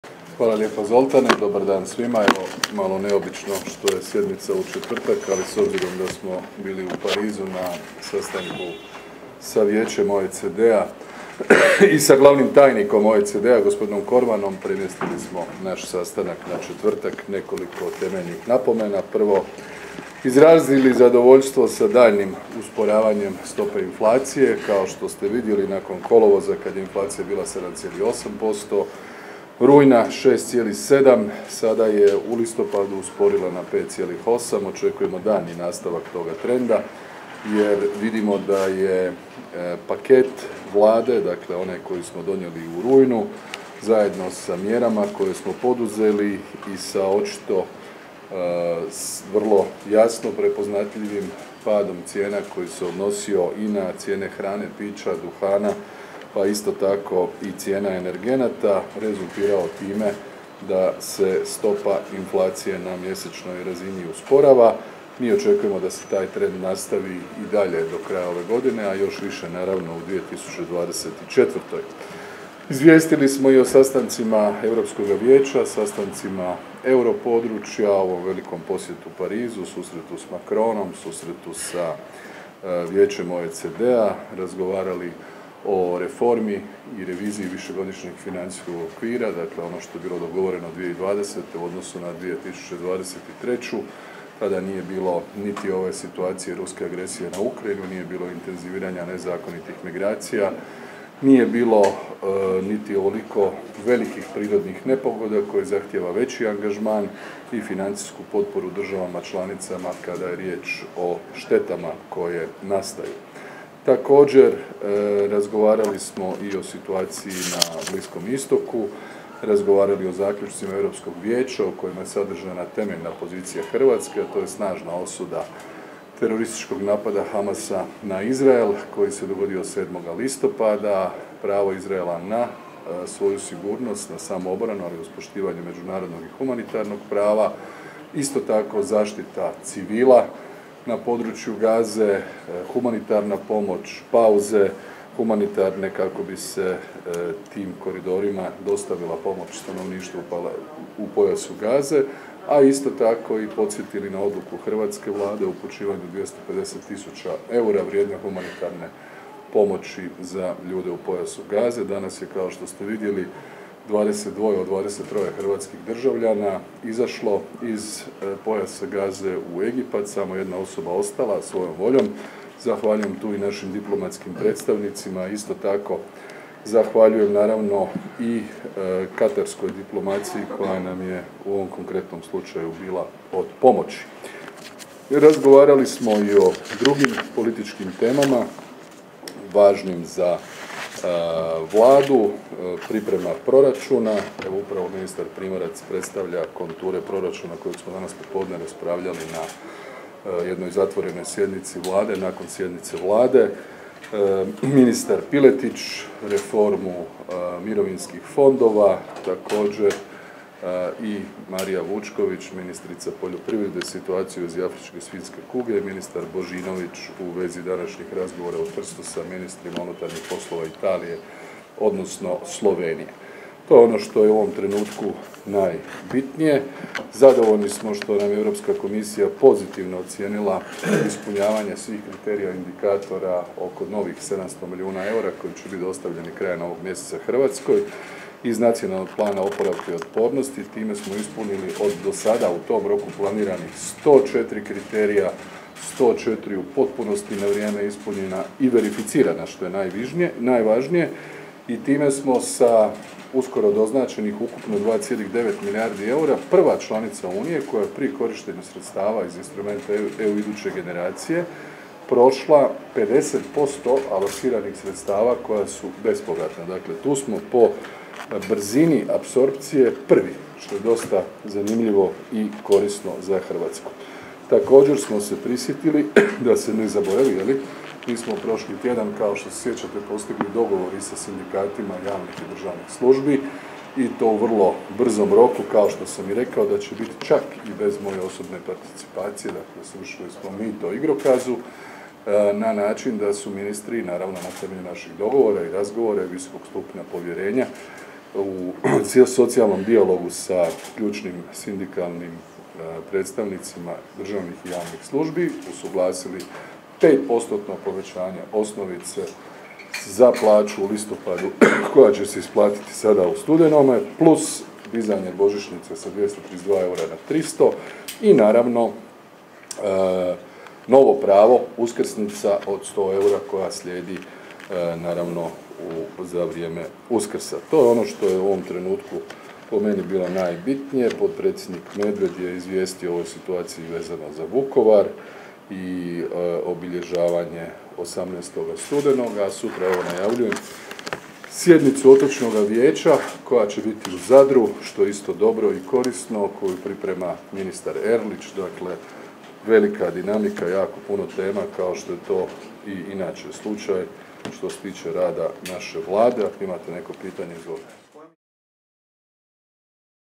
Plenković: Ne podržavamo rezoluciju koja ne osuđuje teroristički napad na Izrael
Zagreb, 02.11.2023. - Predsjednik Vlade Andrej Plenković, obraćajući se novinarima kazao je kako Hrvatska ne podržava neobvezujuću rezoluciju UN-a u kojoj se ne osuđuje teroristički Hamasov napad na Izrael.
video HINA/ Edvard ŠUŠAK/ es